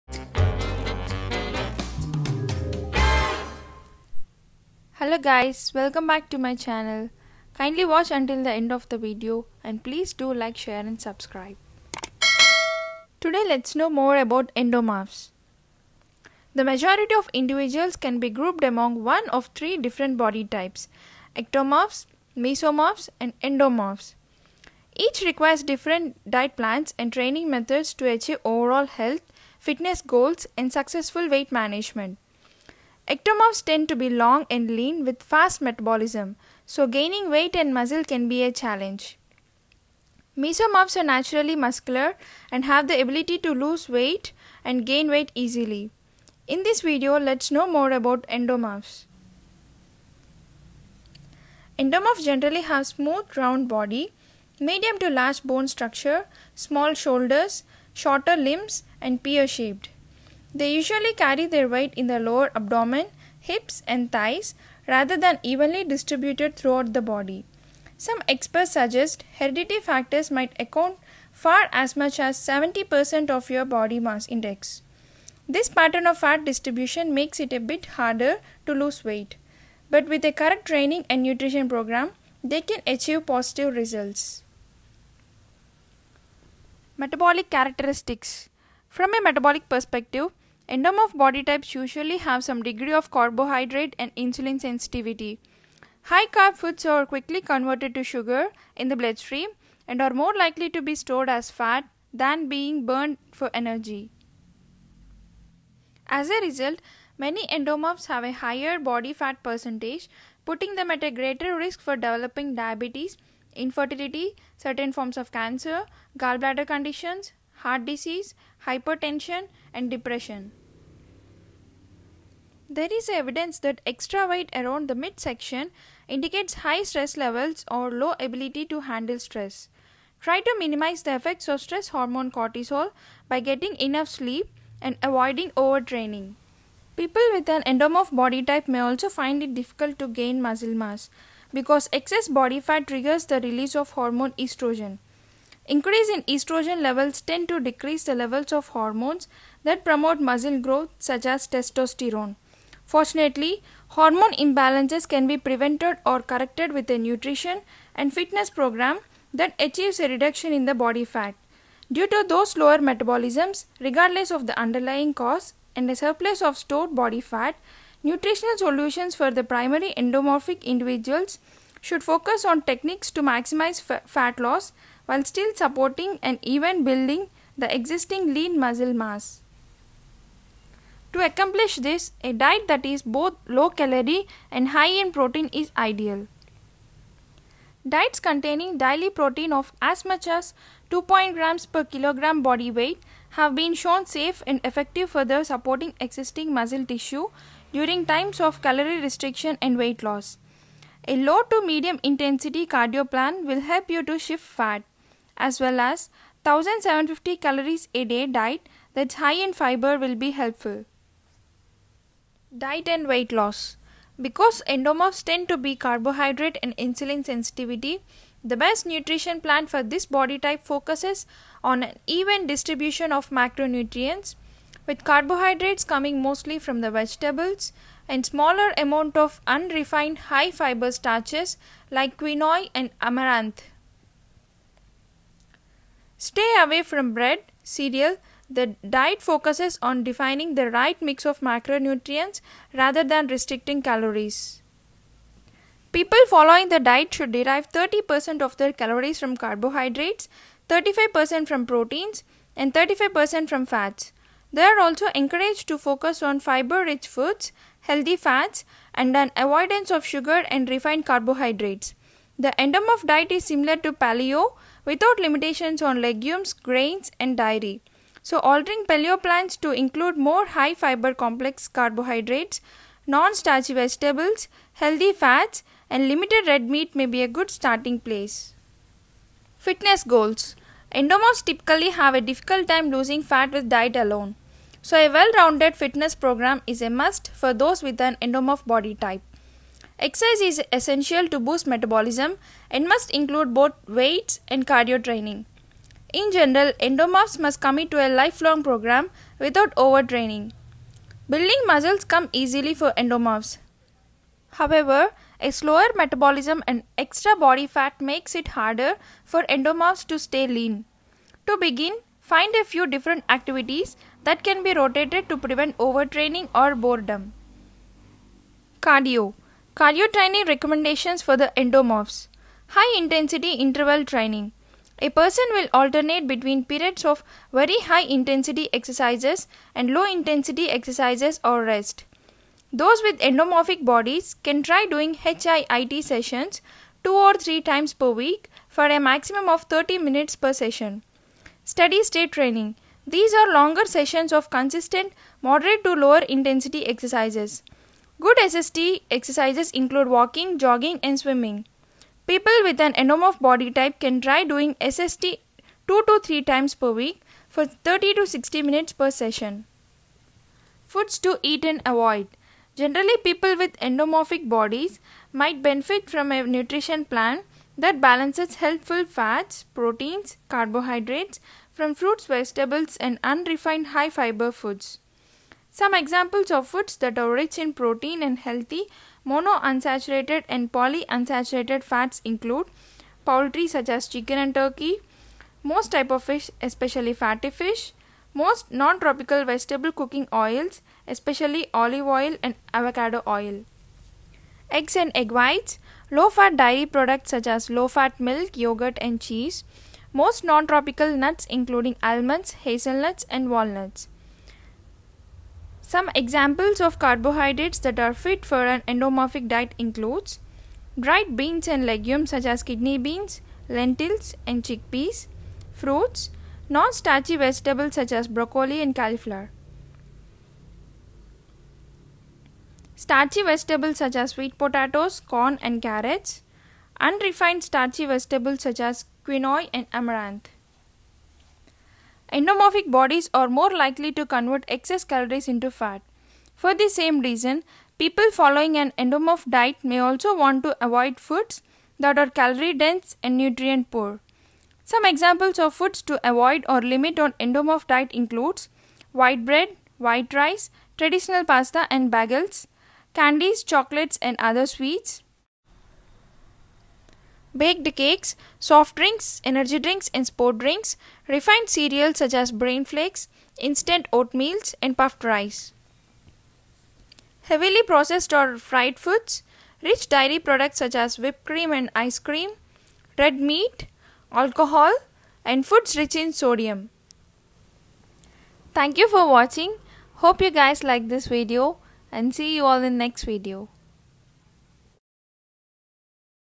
What is ENDOMORPH body type ? How to lose weight as an ENDOMORPH ? | SR Medbrains
Know what is endomorph body type, foods to eat and avoid for endomorph diet. Every single person has a specific body type. Among all Endomorphs have hard time to lose weight. Knowing your body type can greatly help to lose weight as each requires different diet plans and training methods to achieve overall health.